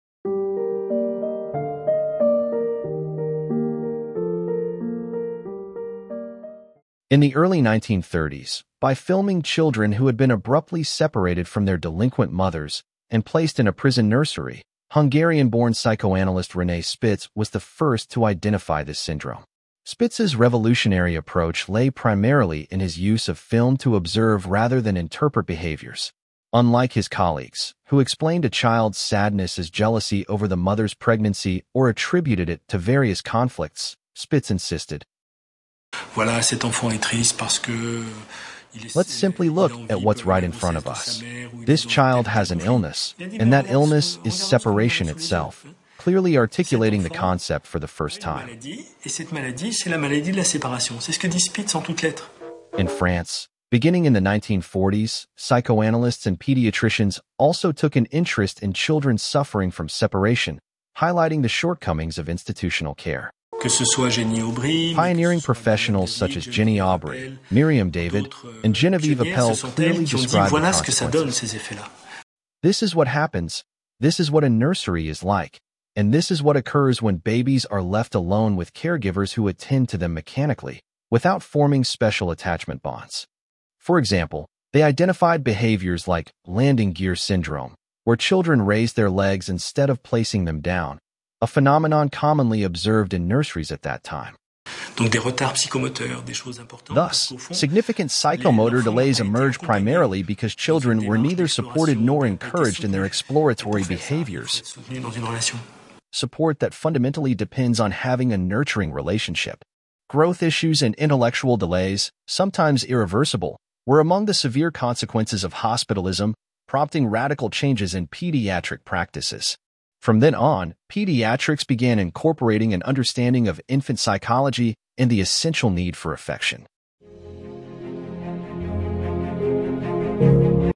Child Psychology, Attachment Issues & Institutional Care: René Spitz and the Origins of Hospitalism
In the early 1930s, Hungarian-born psychoanalyst René Spitz conducted groundbreaking research by filming children abruptly separated from their delinquent mothers and placed in prison nurseries. Spitz's innovative use of observational film allowed him to identify the syndrome known as "hospitalism," emphasizing separation itself as an illness.

By the 1940s, French psychoanalysts and pediatricians such as Ginny Aubrey, Miriam David, and Genevieve Appel further examined the impacts of institutional care on separated children. Their findings revealed significant developmental issues stemming from caregivers performing tasks mechanically, without creating meaningful attachment bonds.

One identified phenomenon was "landing gear syndrome," in which infants raised their legs instead of placing them down—a clear indicator of psychomotor delay. Researchers recognized that these developmental setbacks occurred primarily due to the absence of nurturing support during crucial exploratory stages.

These insights highlighted severe growth and intellectual delays, sometimes irreversible, compelling radical reforms in pediatric care. The emerging approach integrated infant psychology and emphasized affection as vital to child development.

Read "Living to the power of two":